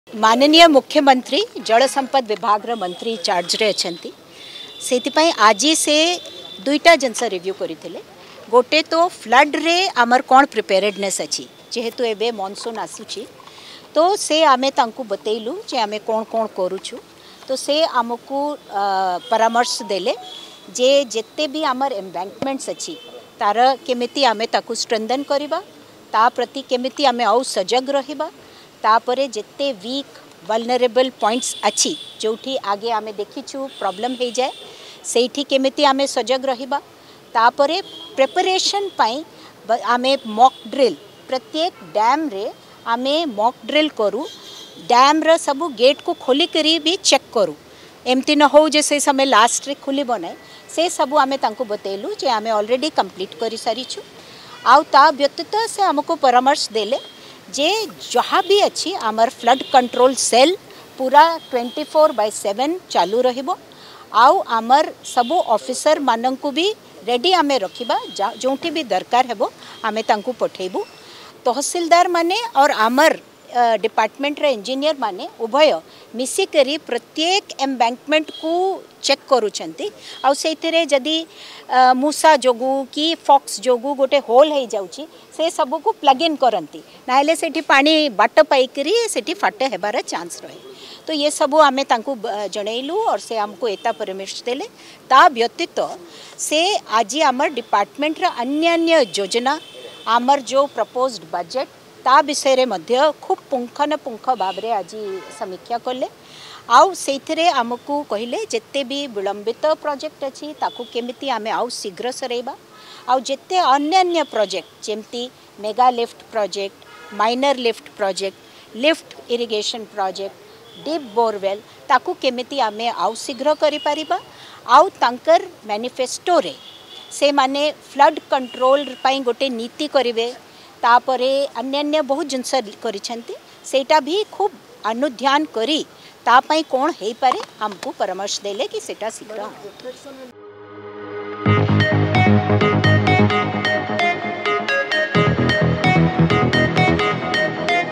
`ବିଳମ୍ବିତ ପ୍ରକଳ୍ପକୁ ଶେଷ କରିବାକୁ ନିର୍ଦ୍ଦେଶ ଦେଲେ’ | Odisha Reporter
ମୁଖ୍ୟମନ୍ତ୍ରୀ ଜଳସଚେନ ବିଭାଗର ସମୀକ୍ଷା କରିବା ପରେ ବିଭାଗୀୟ ସଚିବ ଅନୁ ଗର୍ଗ ପ୍ରତିକ୍ରିୟା ଦେଇ କହିଛନ୍ତି। ବନ୍ୟାକୁ ନେଇ ବିଭାଗ କେତେ ପ୍ରସ୍ତୁତ ସେ ନେଇ ଆଜି ମୁଖ୍ୟମନ୍ତ୍ରୀ ସମୀକ୍ଷା କରିଛନ୍ତି। ଦୁର୍ବଳ ନଦୀ ବନ୍ଧ ଗୁଡିକ ଅଧିକ ସୁଦୃଢ କରିବା ଓ ସେଥିପାଇଁ ସଜାଗ ରହିବାକୁ ମୁଖ୍ୟମନ୍ତ୍ରୀ ନିର୍ଦ୍ଦେଶ ଦେଇଛନ୍ତି ।